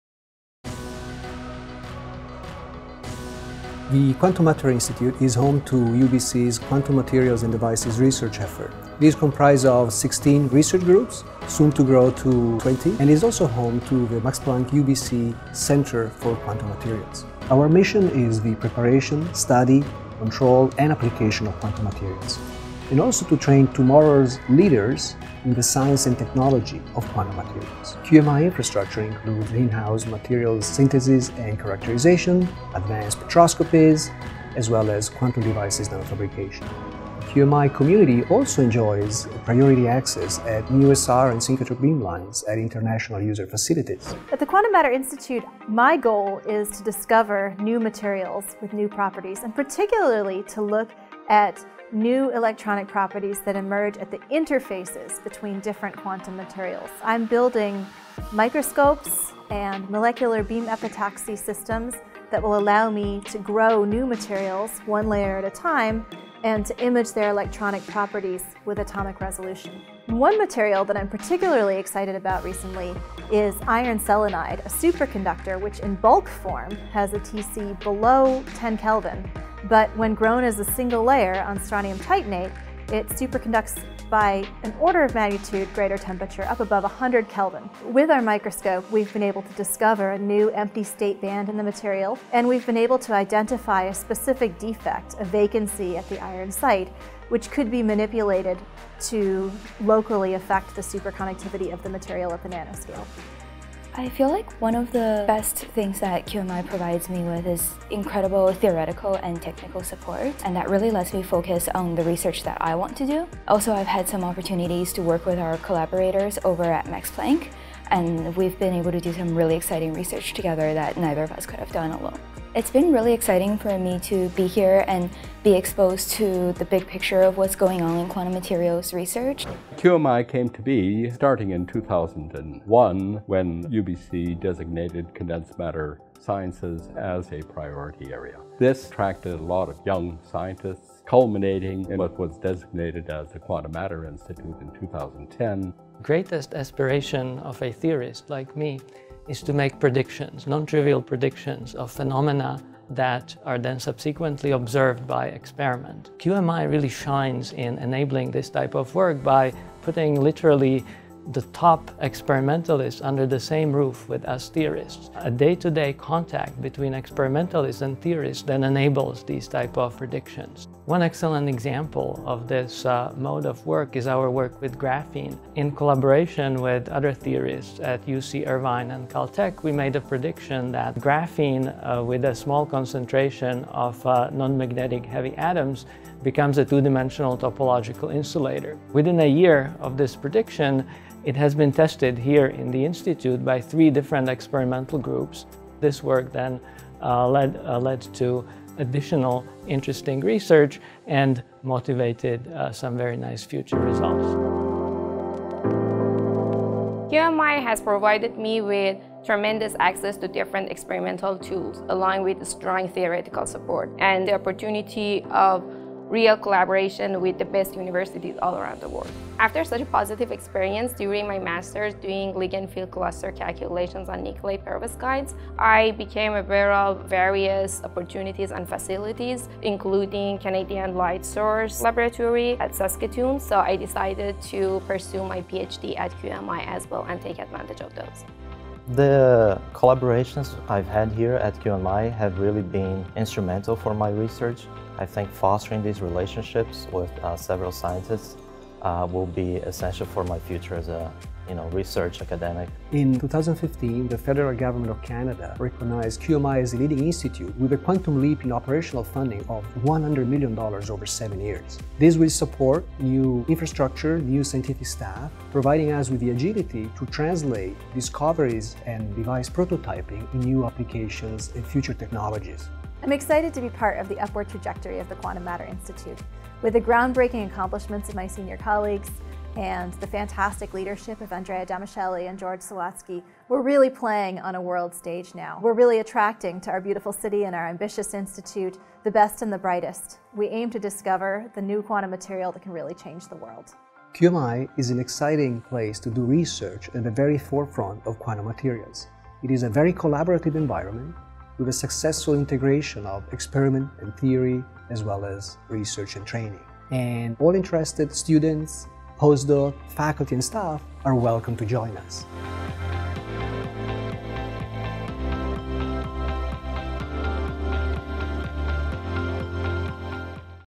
Stewart Blusson Quantum Matter Institute, University of British Columbia
The Quantum Matter Institute comprises a critical mass of internationally renowned investigators (16 faculty, to become 20 with 4 new ongoing hires) and builds upon a long-standing quantum materials research cluster at the University of British Columbia that is recognized as among the best in the world. QMI has had recurring success in attracting external research funding, and priority support from UBC.

QMI’s research excellence was recognized in 2012 with the establishment of the Max Planck-UBC Centre for Quantum Materials and in 2014 with the establishment of the UBC Canada Excellence Research Chair in Quantum Materials and Devices. The QMI faculty has also benefitted tremendously from the few decade long connection with the Quantum Materials Program of the Canadian Institute for Advanced Research, and more recently with the EPiQS program of the Gordon and Betty Moore Foundation (two QMI faculty are Moore funded EPiQS investigators).